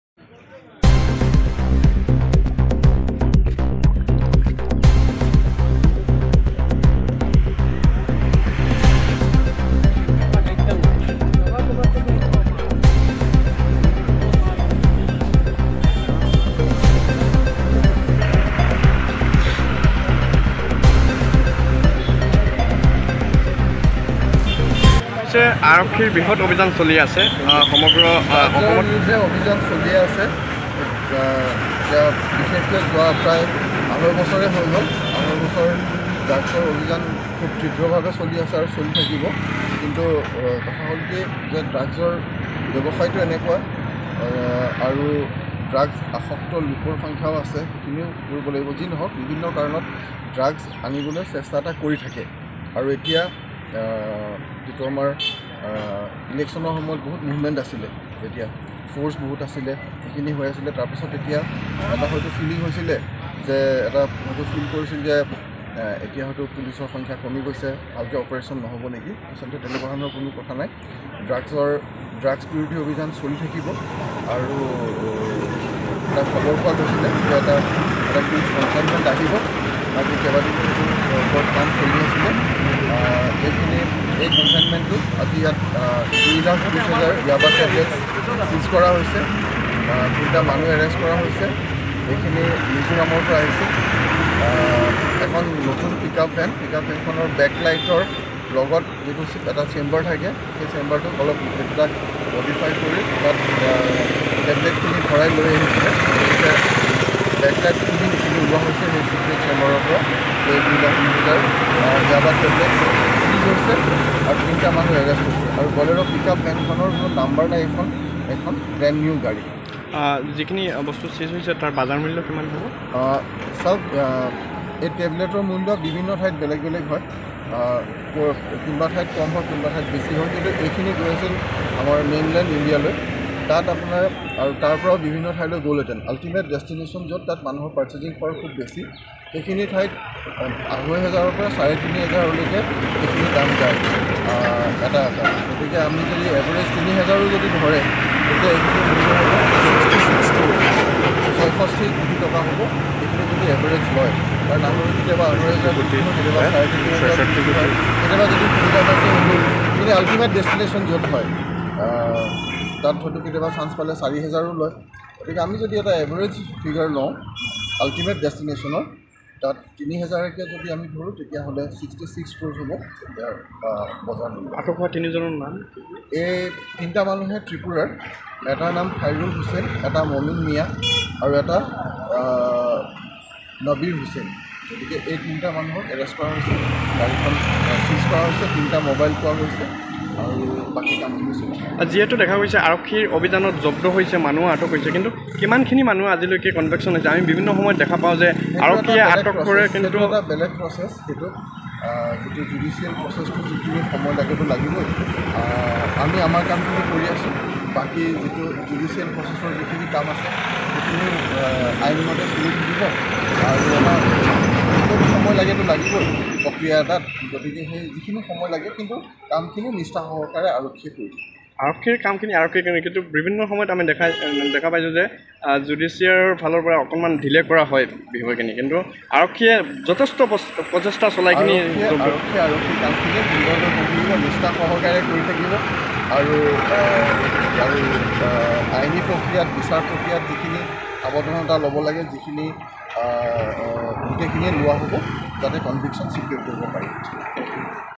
66 Crore Ni Yaba Tablet Manjakha || Tripura Ni Drugs Mafia Khoroktham No Romkha Karimganj Police
Hukumu TV